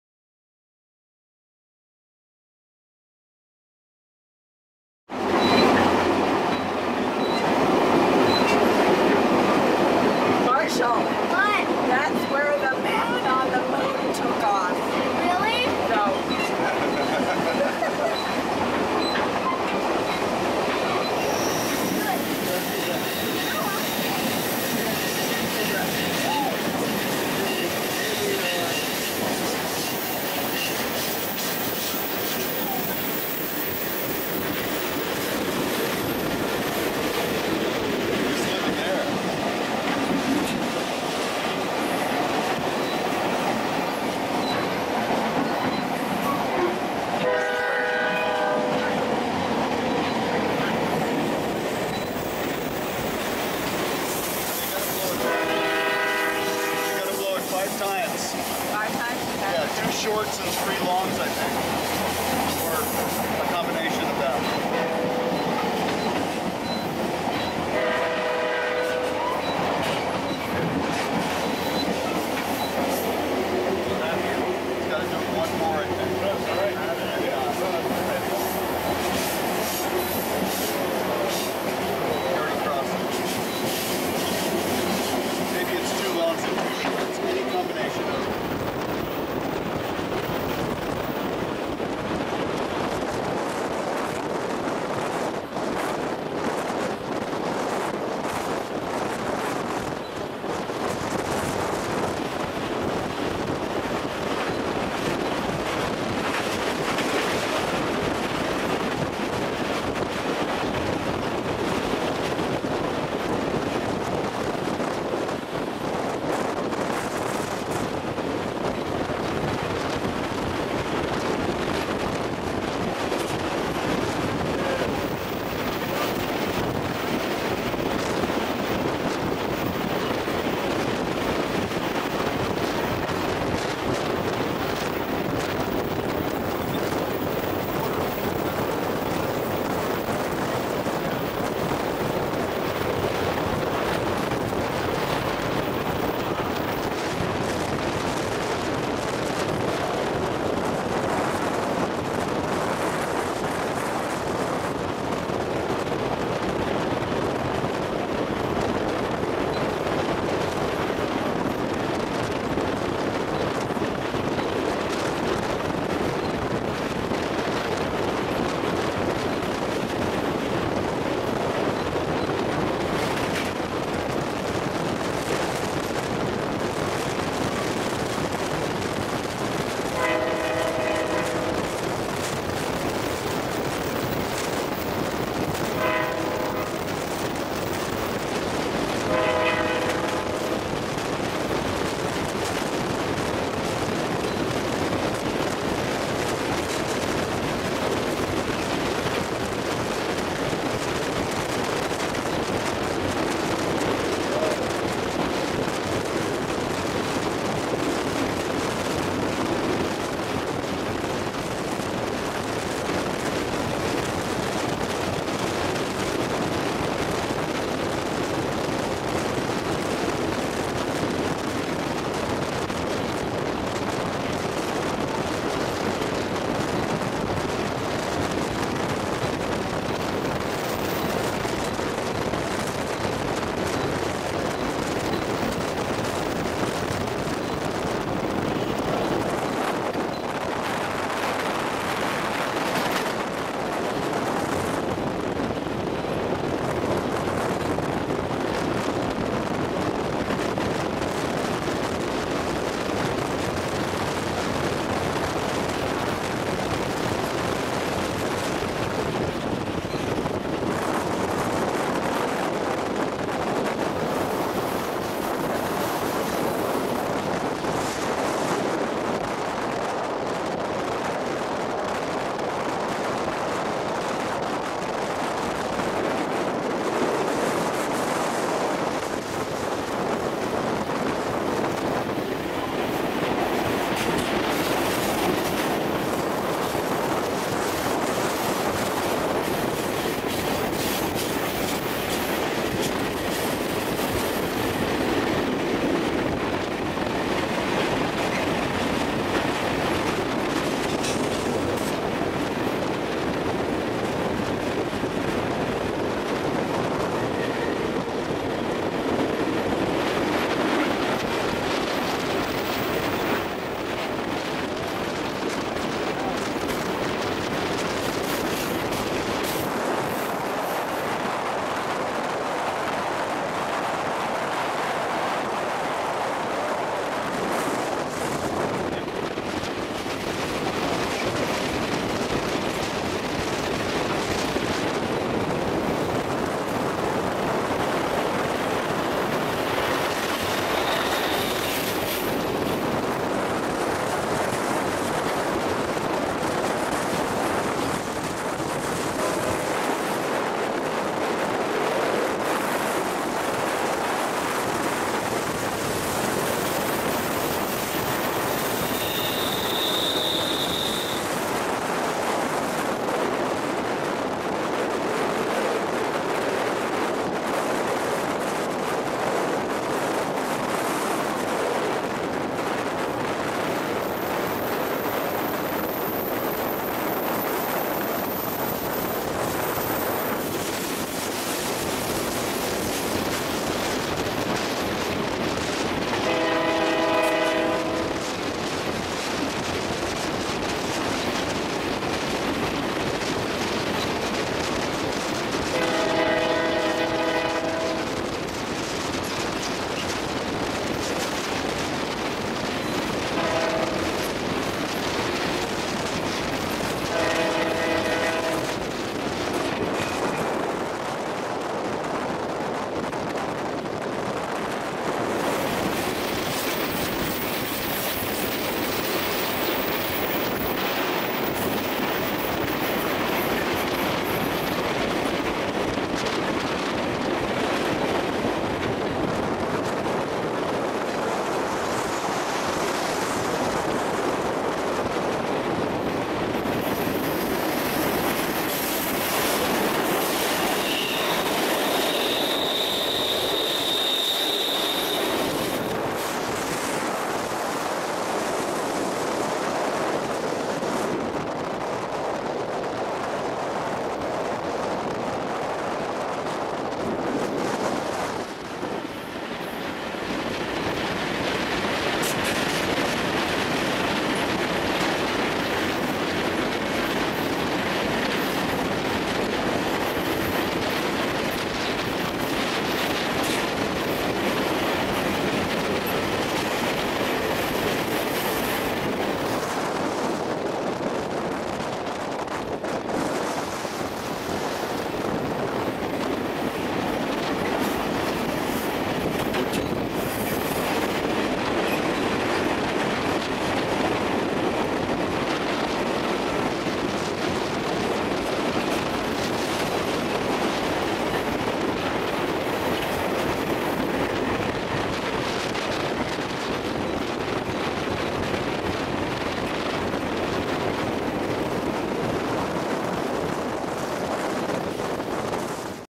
Private Car Silver Splendor Vestibule Ride Part 1!!!
On April 7, 2012, I was fortunate enough to ride the private car Silver Splendor again up to San Luis Obispo and back down to Santa Barbara.  This time thought from Surf Beach to past the Vista Point along Highway 101, I hung out around the open vestibule and filmed the scenery along the way.  Highlights in this video include a launch pad at VAFB, Point Arguello, the Sudden Siding (with empty windmill flats in storage), Jalama Beach, Point Conception, along with the beautiful beaches, the coast, and the Pacific Ocean.  Apologies about the wind in the video as it is quite breezy North of Point Conception.